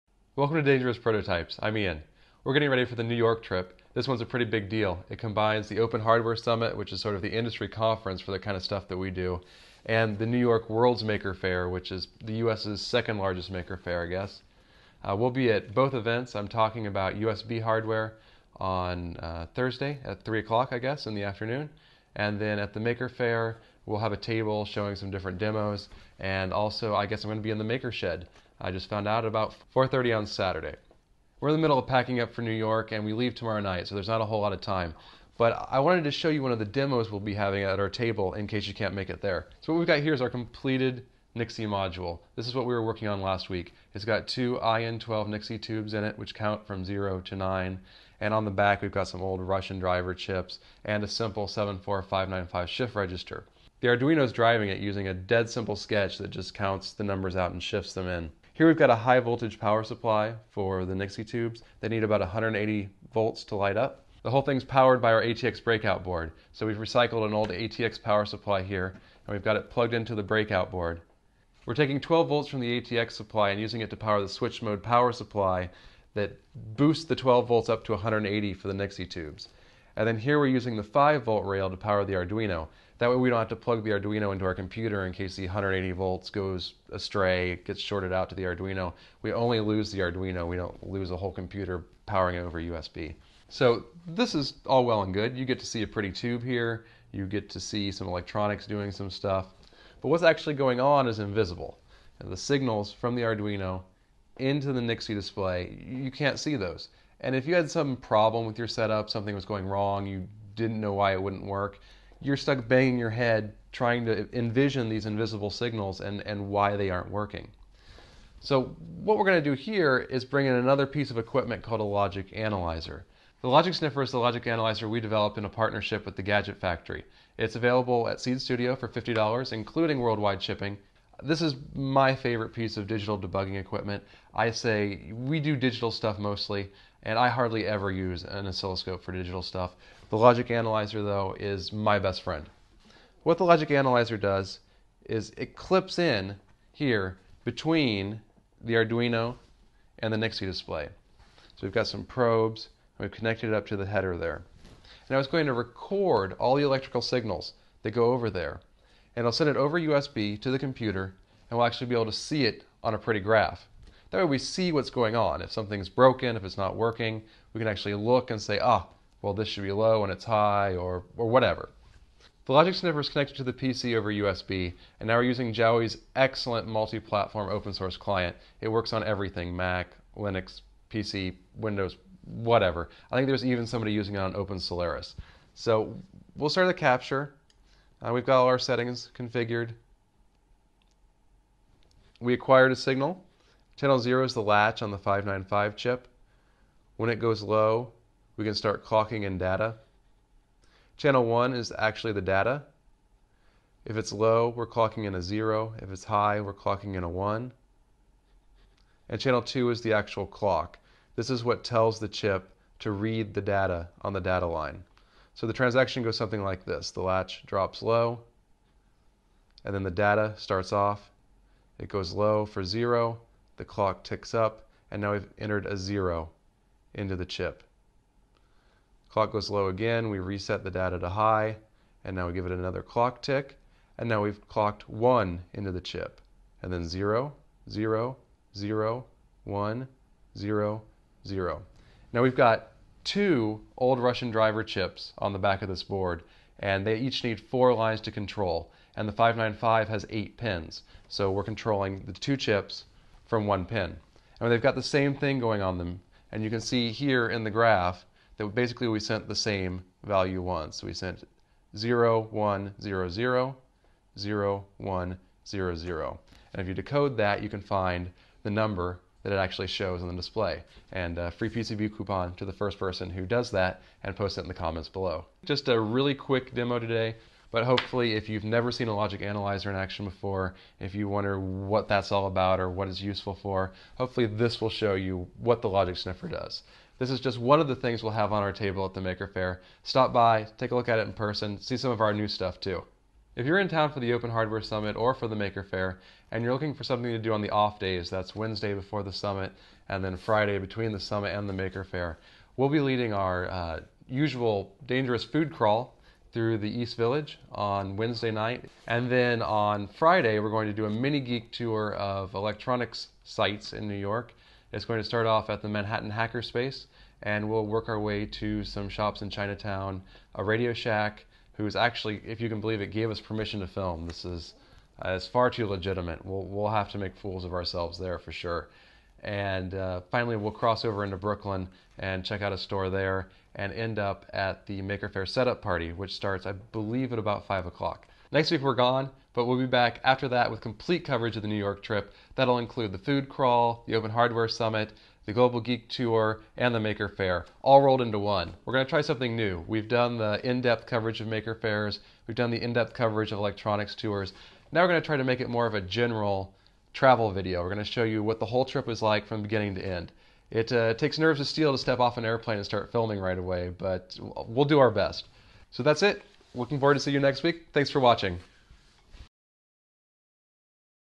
WORKSHOP VIDEO #40: Nixie tube and logic analyzer demo for Maker Faire
We're getting ready for the Global Geek Tour of New York. For those of you who can't join us at the World Maker Faire, here's a quick logic analyzer demo using a single block of the Magnificent Nixie Thing we're tinkering with.